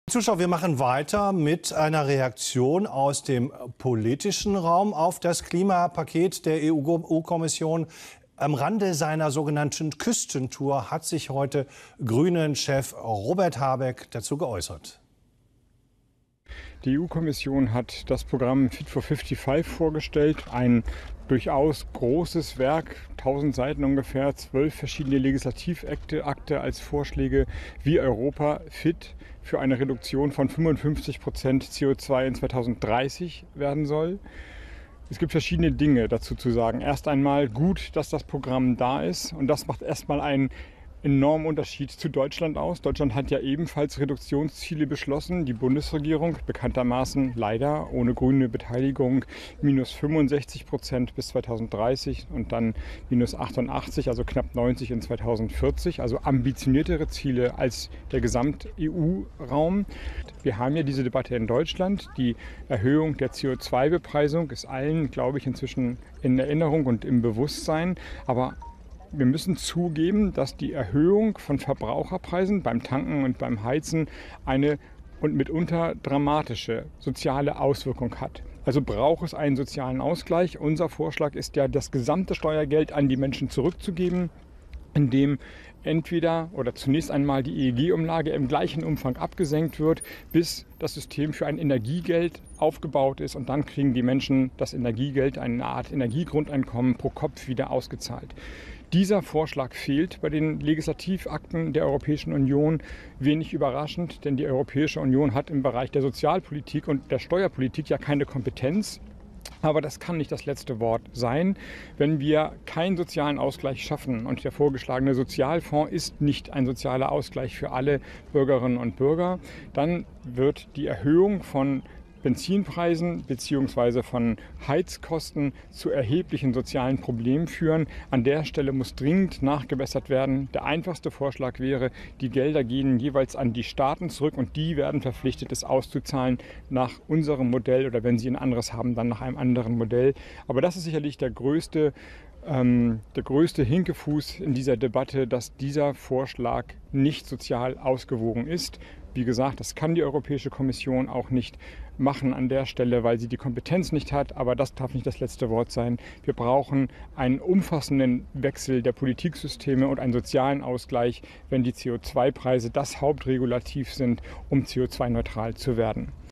Reaktion von Robert Habeck (B'90/GRÜNE, Parteivorsitzender) auf "Fit for 55" am 14.07.21
Reaktion von Robert Habeck (B'90/GRÜNE, Parteivorsitzender) auf das neue große Klimapaket "Fit for 55" der EU